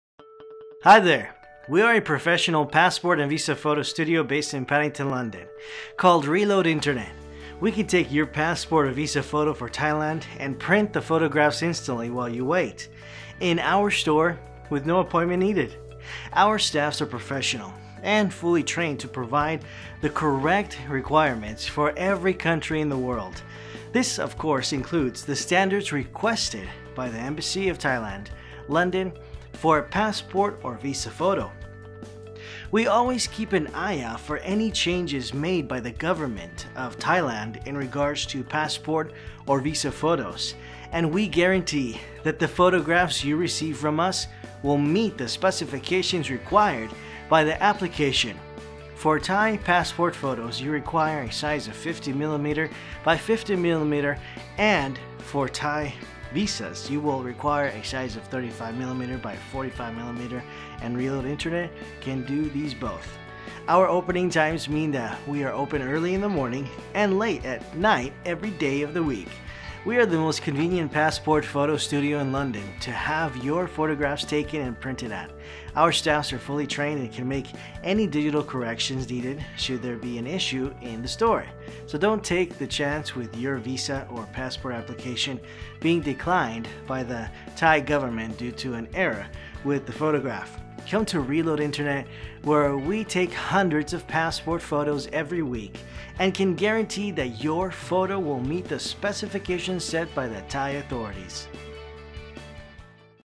Thai Passport Photo and Visa Photo snapped instantly in Paddington, London, at Reload Internet
Reload Internet is a passport photo studio in Paddington, London, that can capture, print and prepare your Thai passport or visa photos instantly in our store.

 Our staff have been professionally trained to provide you with a set of high quality photographs meeting the exact requirements stated by the Embassy of Thailand. We are open 7 days a week at Reload Internet, from early in the morning until late into the evening, so you can visit us to have your Thai passport or visa photograph taken at a time that suits you.

We can guarantee that the photographs you receive from Reload Internet will pass when you submit your Thai passport/visa application. You don't even need an appointment with us, just visit our store and our staff will be happy to help you obtain a Thai passport or visa photograph that meets all of the necessary requirements!

Thai passport and visa photo guidelines are as follows:
• Two identical photographs, sized 45mm x 35mm
• Photos taken at photo booths and with computers will be rejected by the Royal Thai embassy
• A plain white or off-white background should be used, shadows on both the applicants face and background will result in rejection by the Royal Thai embassy
• Your face should cover 70-80% of the entire photo and your facial characteristics must be clearly visible

Our Address is 197 Praed Street, Praed Street, Paddington, W2 1RH
or our number is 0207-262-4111